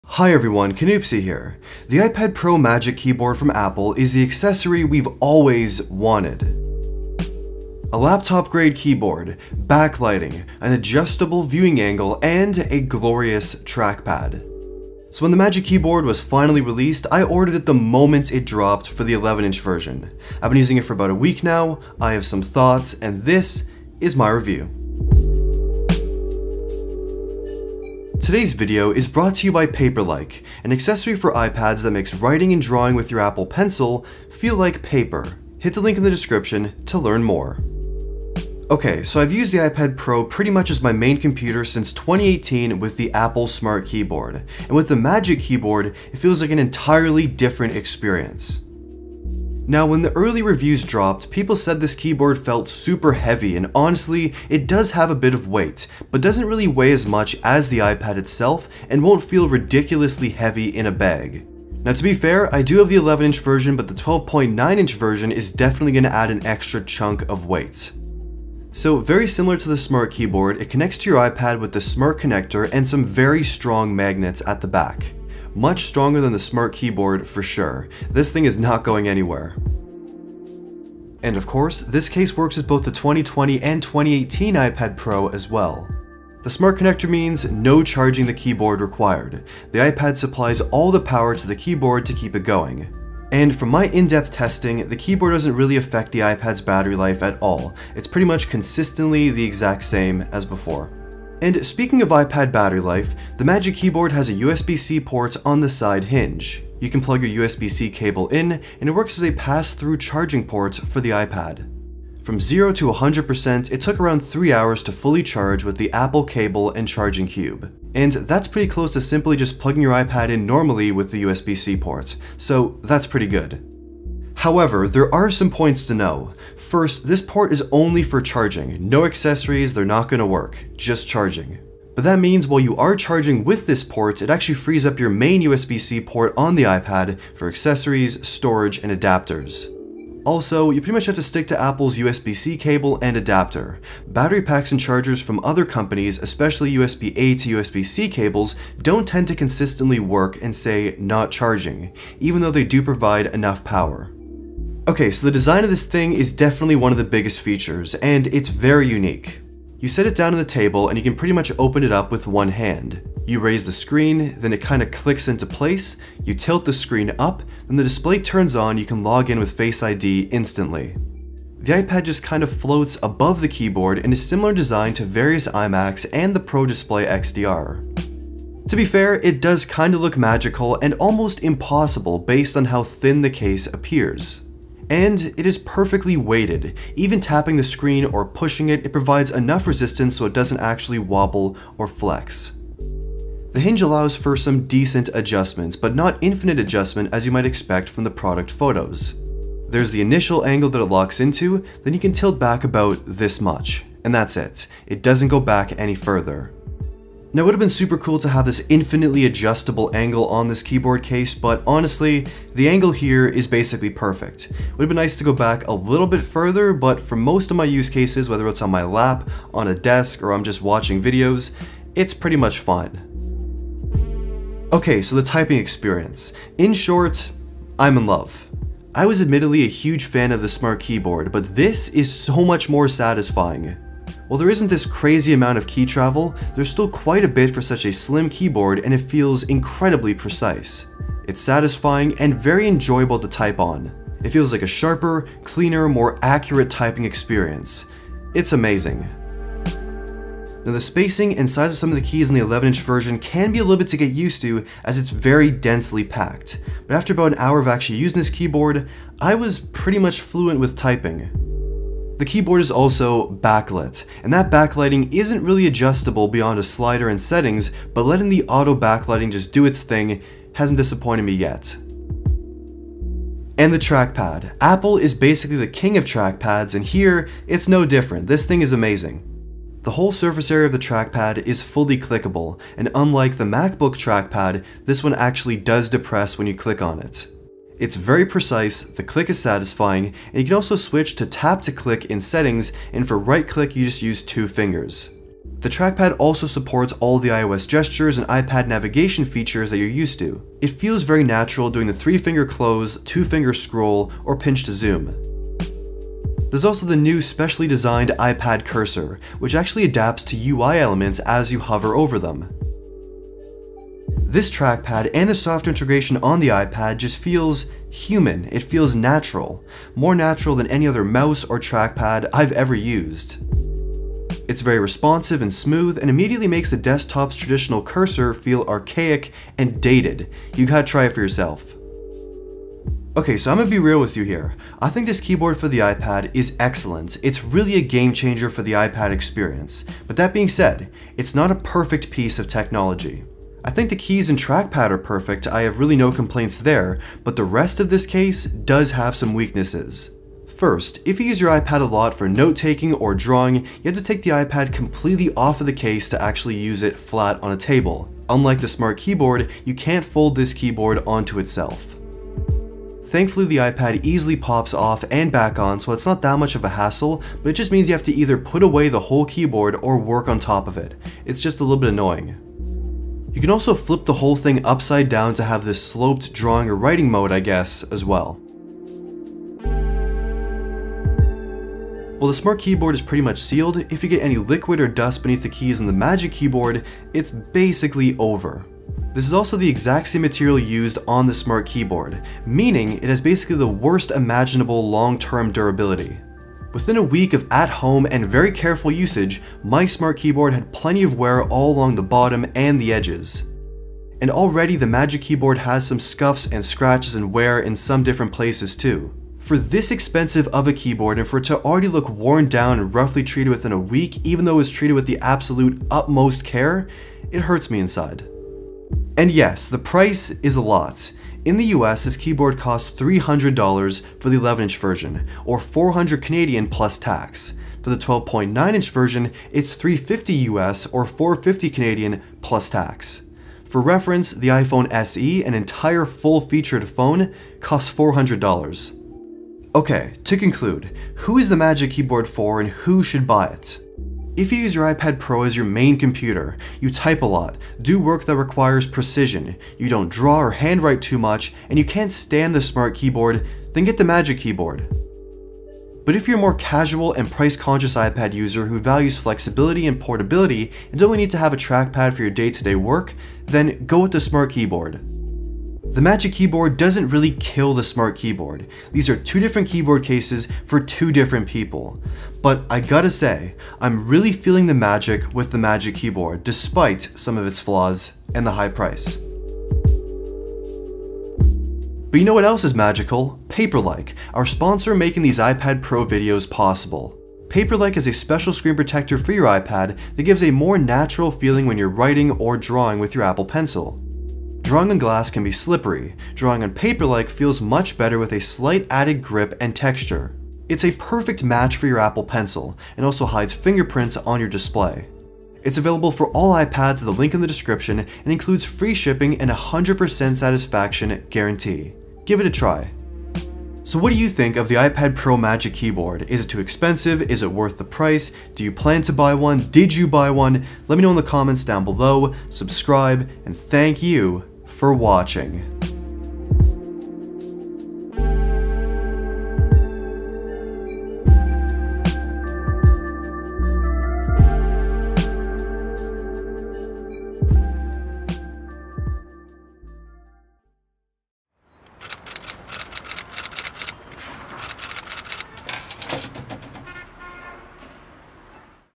iPad Pro Magic Keyboard - The Truth
My iPad Pro Magic Keyboard review and light comparison with the Smart Keyboard from Apple. Is the Magic Keyboard worth it? Who is it for? Find out in this video! (thanks for Paperlike for sponsoring this video!)

Apple iPad Pro Magic Keyboard

My video gear list